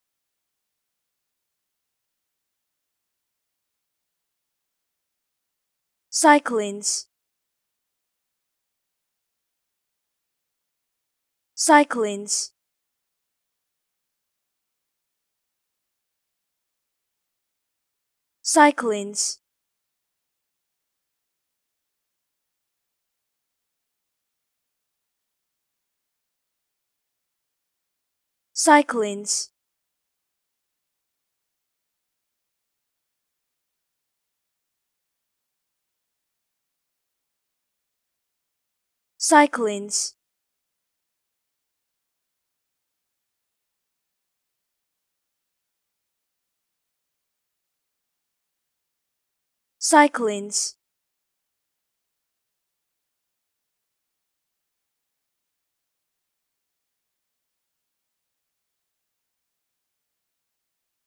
How To Say Cyclins
Learn how to say Cyclins with EmmaSaying free pronunciation tutorials.